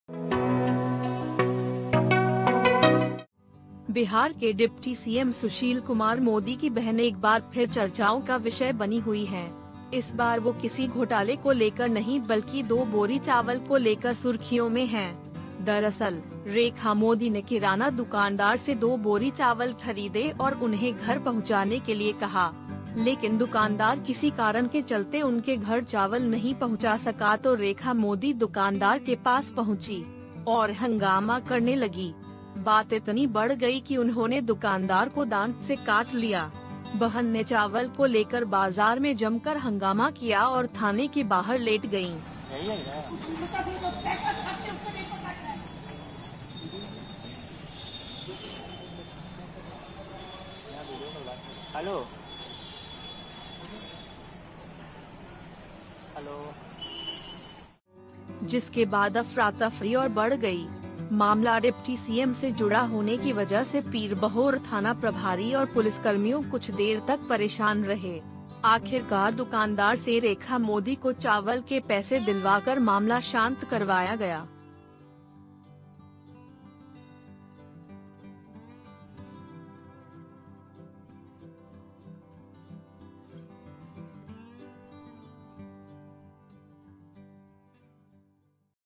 Patna: दो बोरी चावल के लिए थाने में लेटीं डिप्टी सीएम Sushil Modi की बहन Rekha Modi
बिहार के डिप्टी सीएम सुशील कुमार मोदी की बहन एक बार फिर चर्चाओं का विषय बनी हुई हैं... इस बार वो किसी घोटाले को लेकर नहीं बल्कि दो बोरी चावल को लेकर सुर्खियों में हैं.. दरअसल, रेखा मोदी ने किराना दुकानदार से दो बोरी चावल खरीदे और उन्हें घर पहुंचाने के लिए कहा..